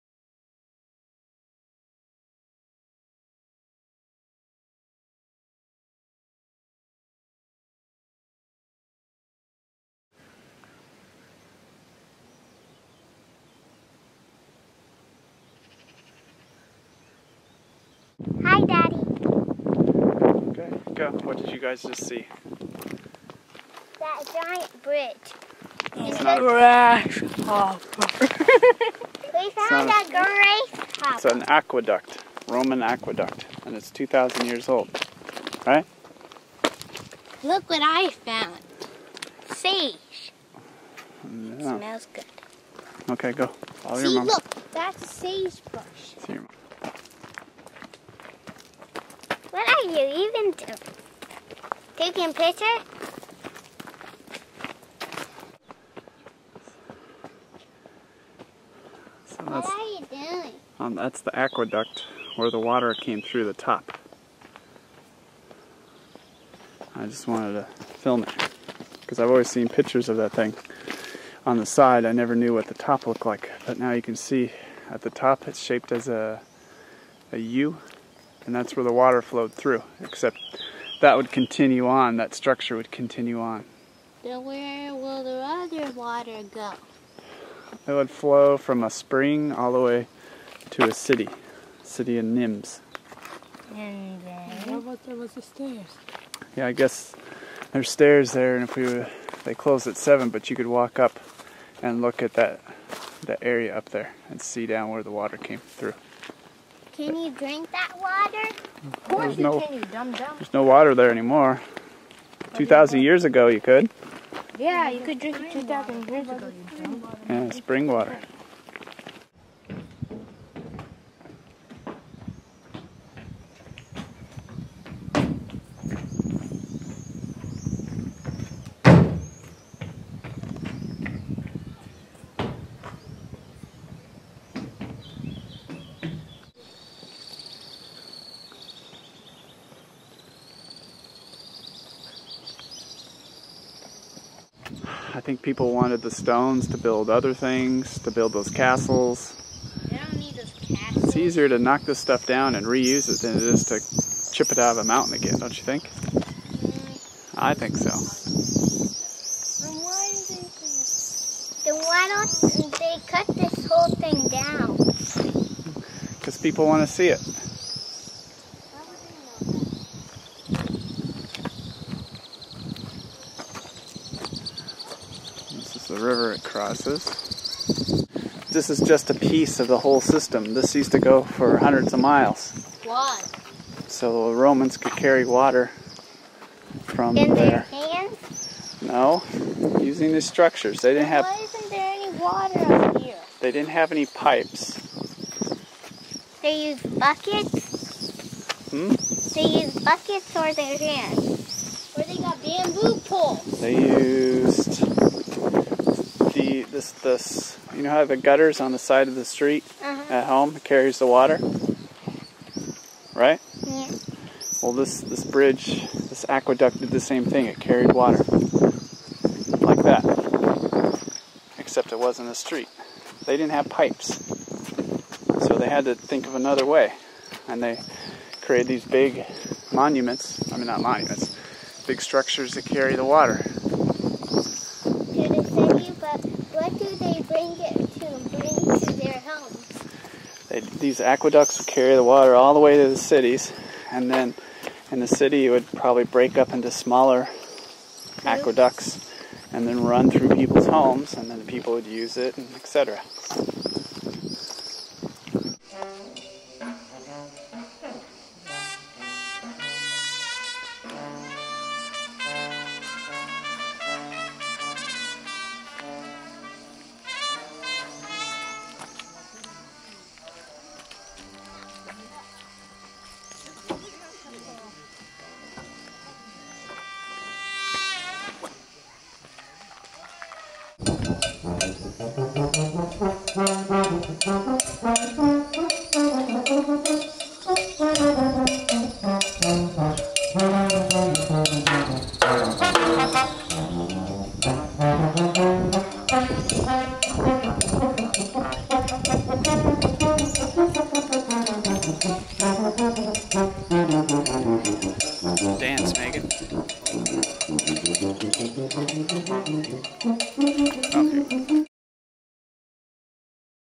2009 Pont Du Gar - France
Hiking around Pont Du Gar with the kids.